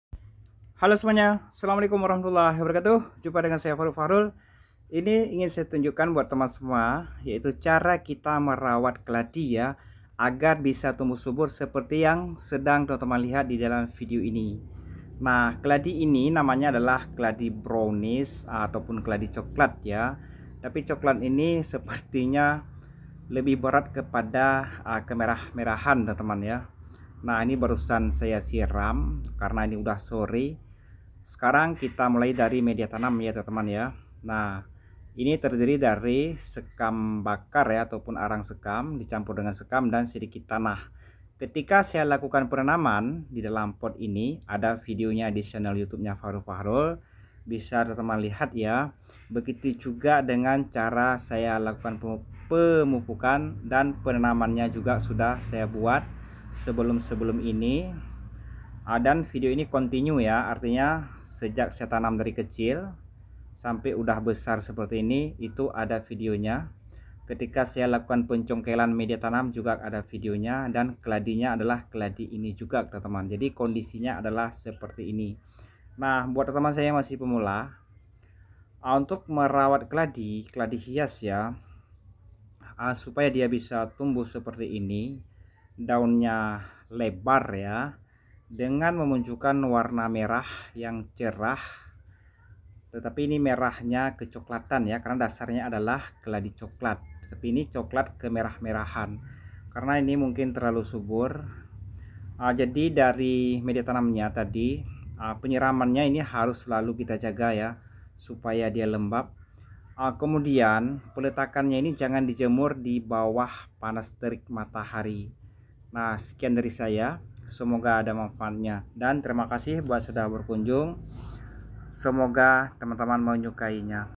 Cara Merawat Keladi Agar Daun Lebar
Tips cara merawat keladi hias agar daun lebar ada beberapa hal yg harus di perhatikan seperti media tanam yang bagus.
Merawat keladi hias supaya subur dan memunculkan warna cerah harus mendapatkan sekitar 50% sinar matahari.